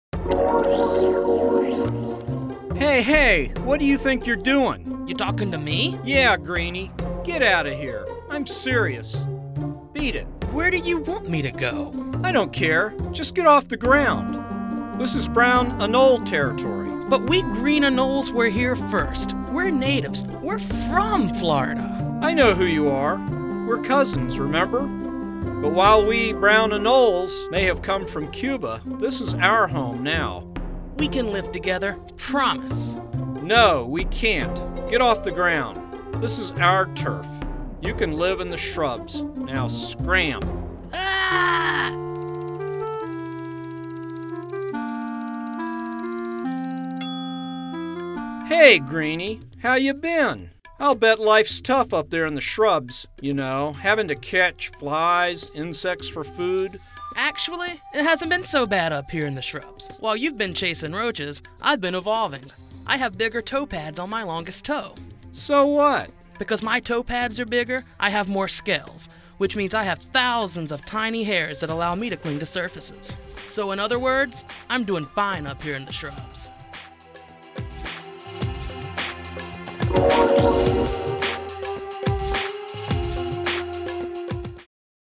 Green vs. Brown: How Anoles Evolved In Florida
Cuban brown anoles have pushed their home-grown Florida cousins, the green anole, off the ground to live in low branches and shrubs. A recent study found that the greens evolved to have bigger toe pads on the longest toe, the fourth.